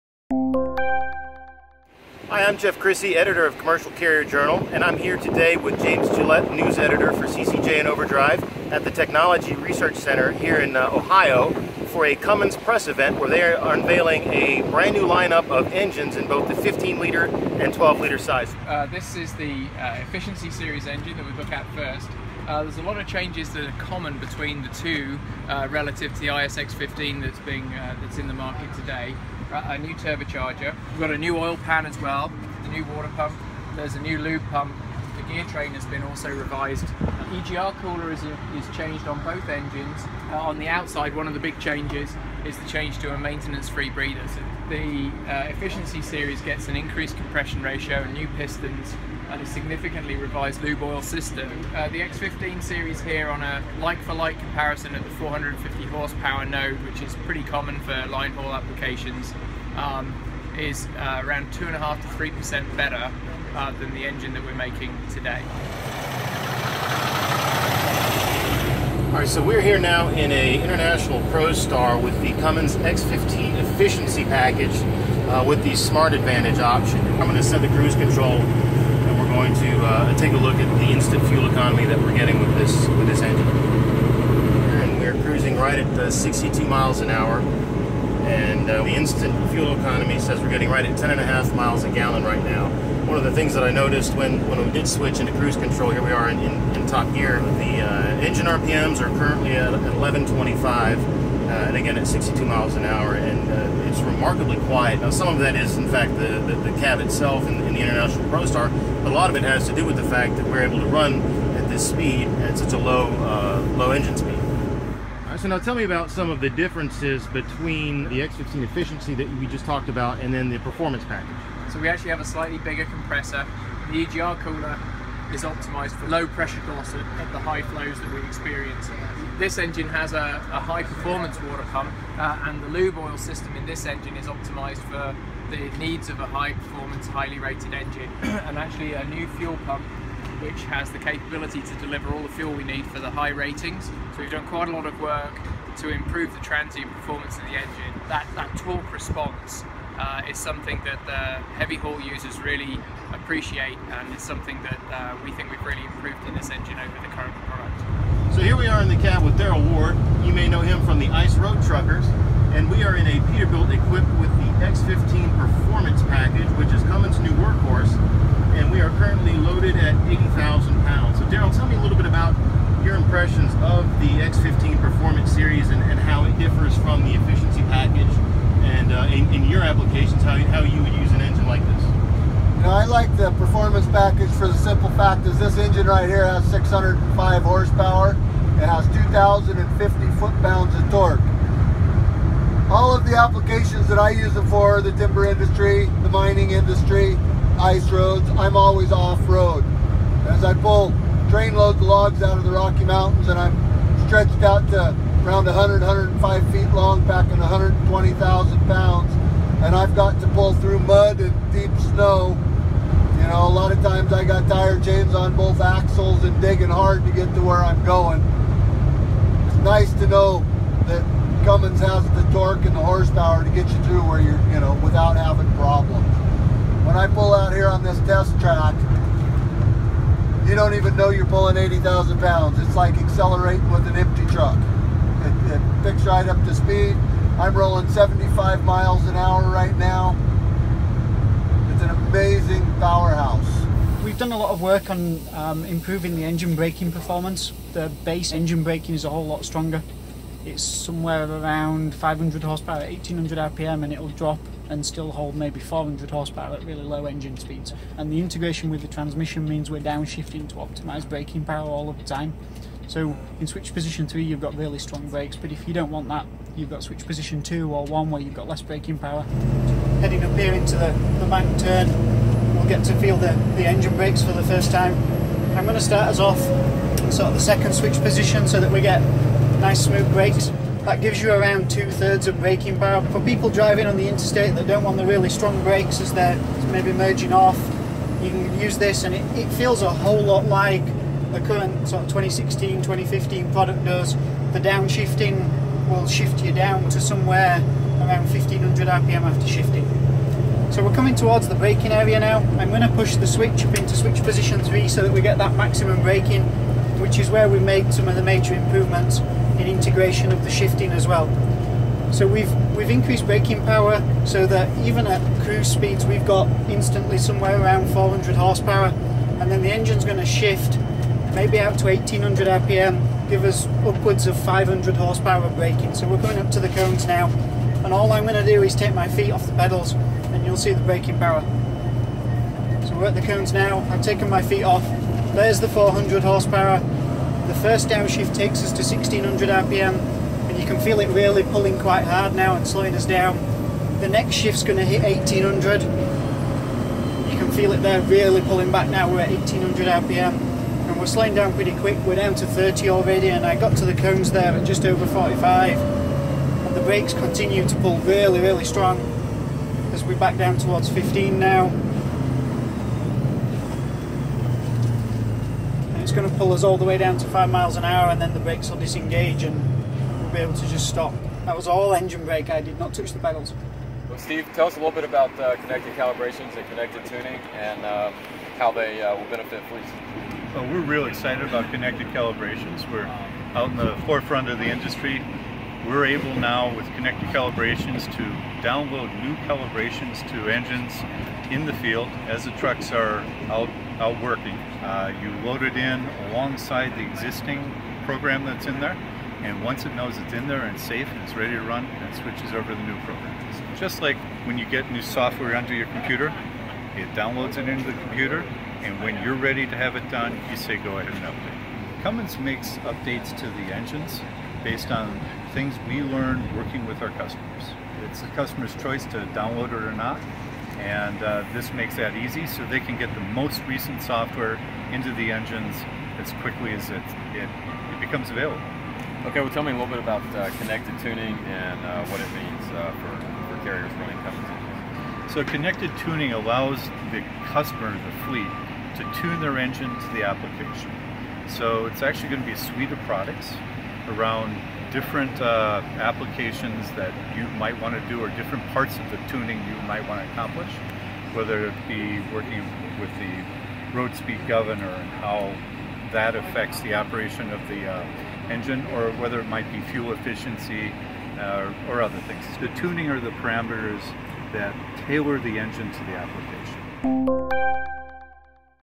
CCJ goes in-depth with Cummins’ new 2017 engines
Cummins introduced in July a brand new, rebranded engine lineup for 2017, highlighted by two new 15-liter engines. The engine maker also introduced a range of other new features and technology upgrades to its heavy-duty offerings. CCJ had a chance to test some of the new features and talk to Cummins reps about what they mean for fleets in a day-long test outside Columbus, Ohio, in July.